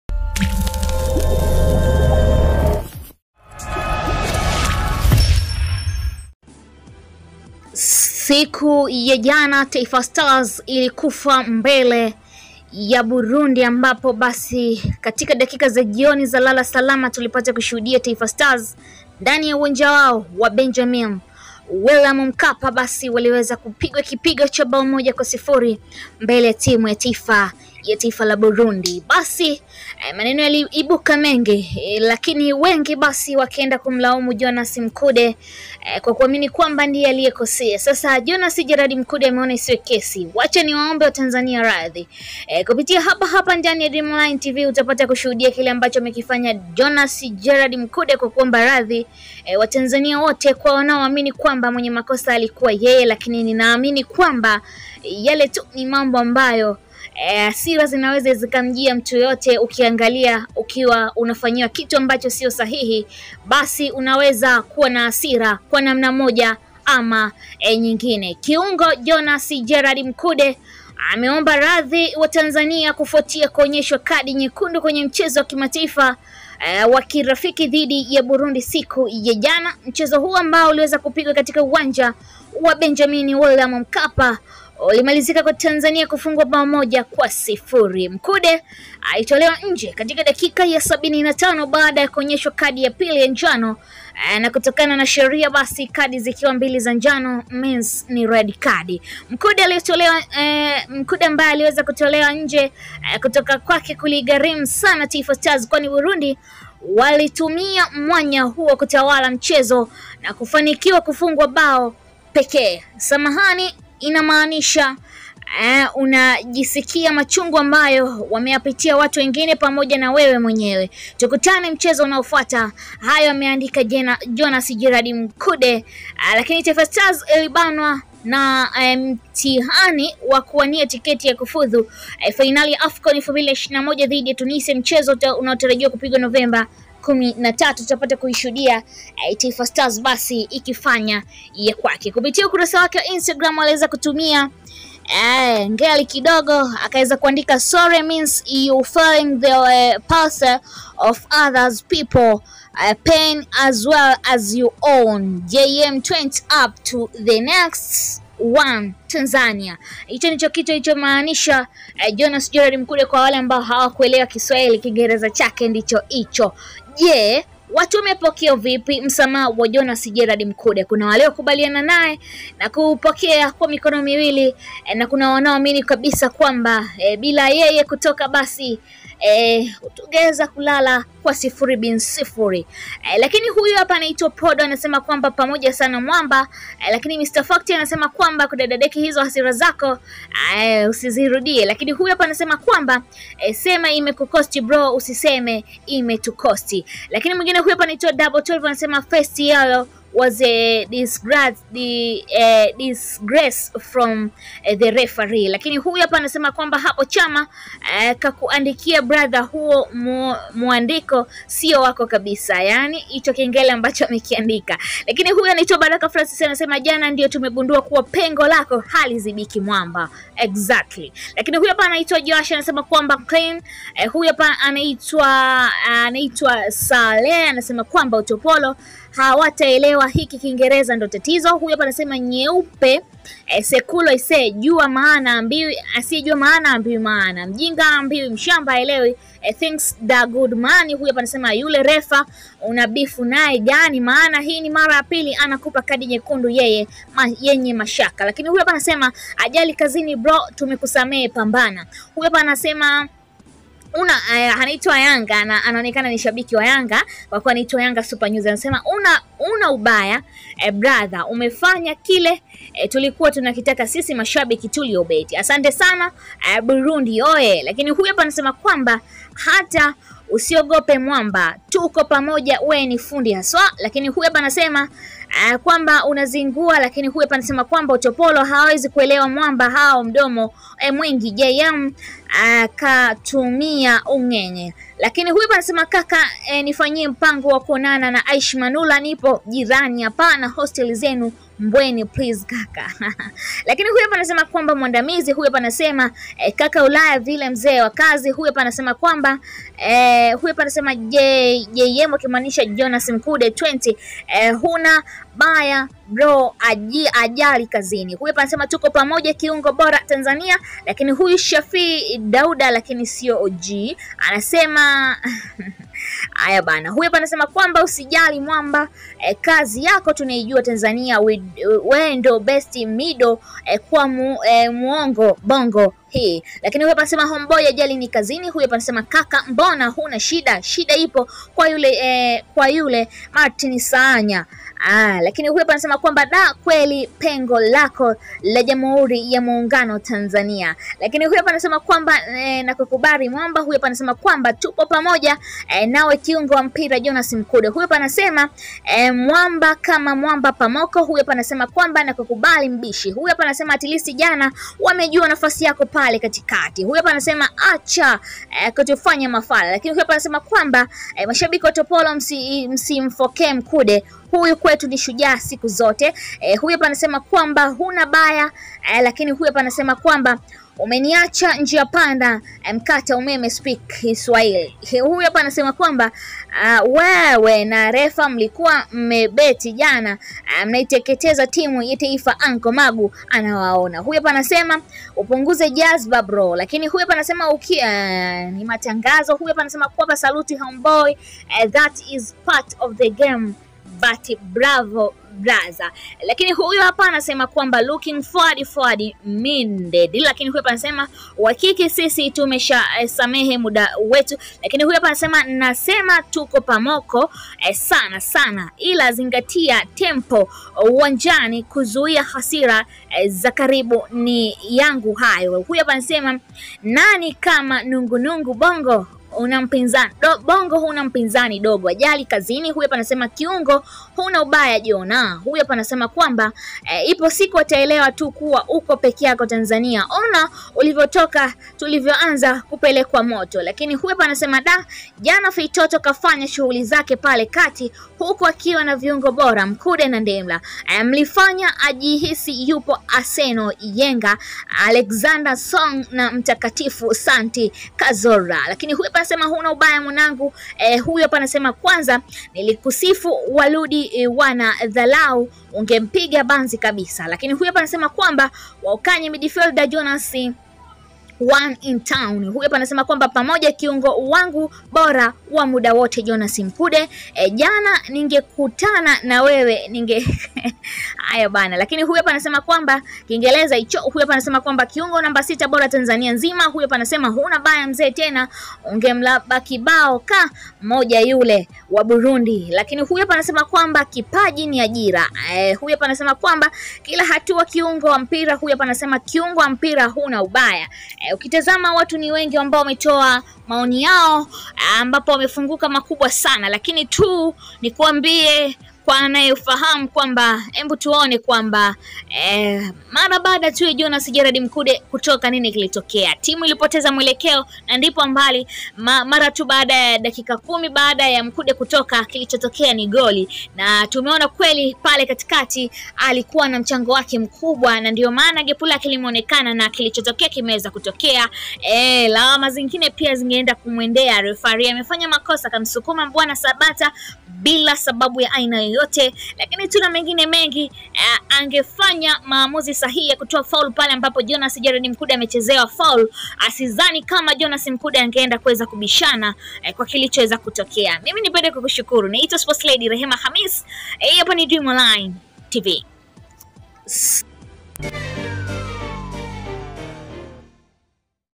JONAS MKUDE AWALILIA WATANZANIA AWAOMBA RADHI KWA KILICHOTOKEA JANA TAIFA "MAUMIVU YANGU NI YENU"
Kiungo Jonas Mkude amewaomba radhi Watanzania kufuatia kuonyeshwa kadi nyekundu kwenye mchezo wa Kimataifa wa kirafiki dhidi ya Burundi jana

Mchezo huo uliopigwa uwanja wa Benjamin Mkapa, ulimalizika kwa Tanzania kufungwa bao 1-0

Mkude alitolewa nje kwenye dakika ya 75, baada ya kuonyeshwa kadi ya pili ya njano

Kutoka kwake kuliigharimu Stars kwani Burundi walitumia mwanya huo kutawala mchezo na kufanikiwa kufunga bao pekee

"Samahani inamaanisha unajisikia machungu ambayo wameyapitia watu wengine pamoja na wewe mwenyewe. Tukutane mchezo unaofuata," aliandika Mkude kwenye ukurasa wake wa mtandao wa kijamii

Stars inakabiliwa na mtihani wa kuwania tiketi ya kufuzu fainali za Afcon 2021 dhidi ya Tunisia mchezo ukitarajiwa kupigwa Novemba 13